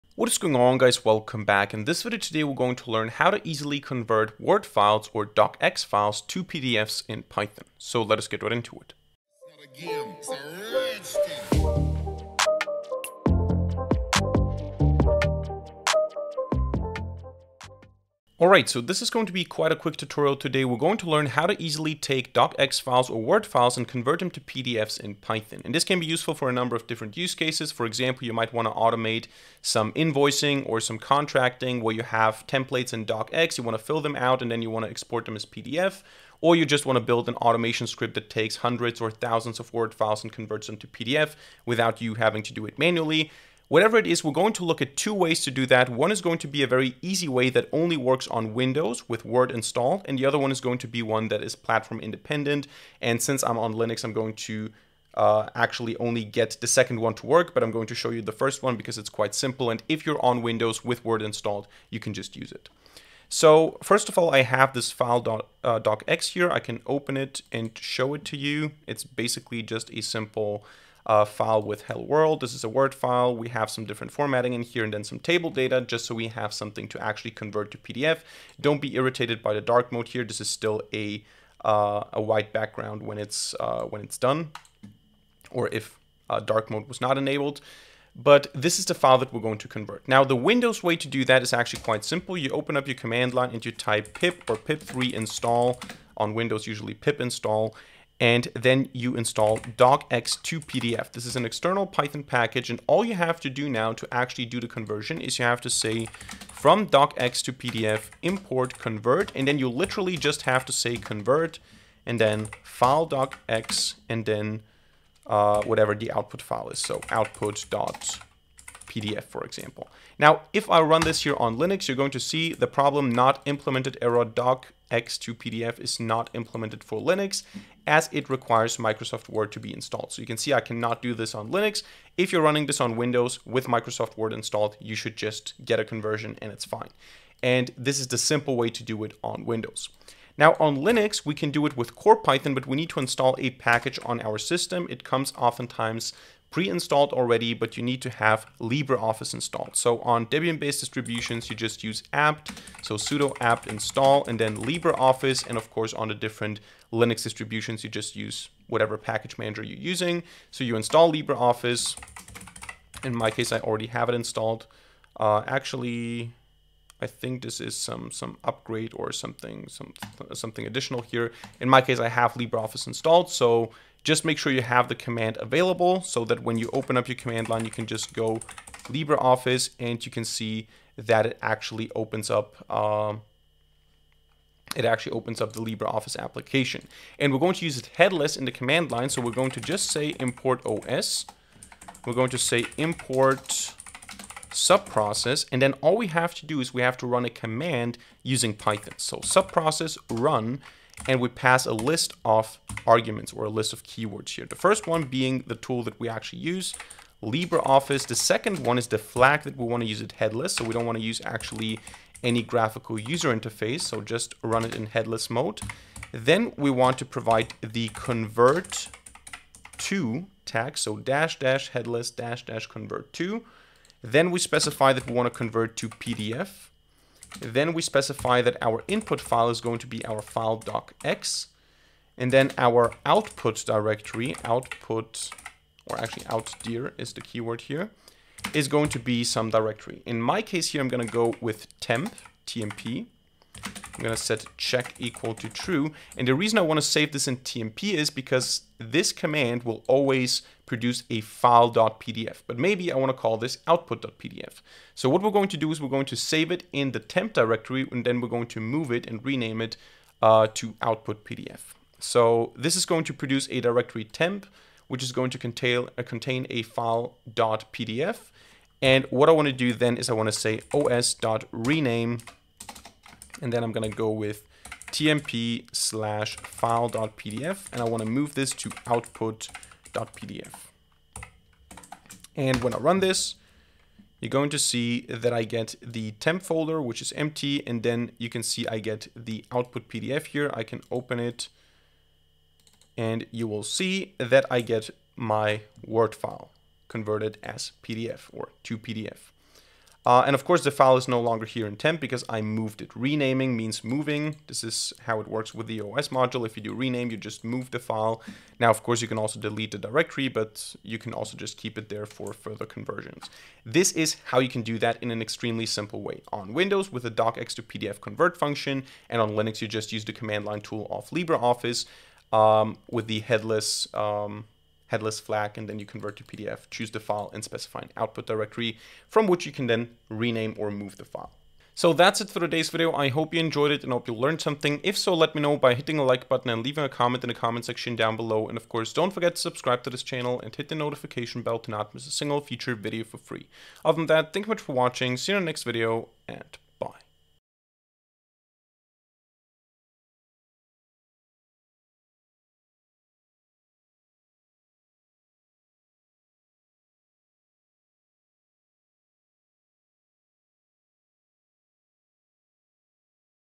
Convert Word Files To PDF in Python - DOCX To PDF
In this video, we learn how to convert Word files (DOCX) to PDFs in Python.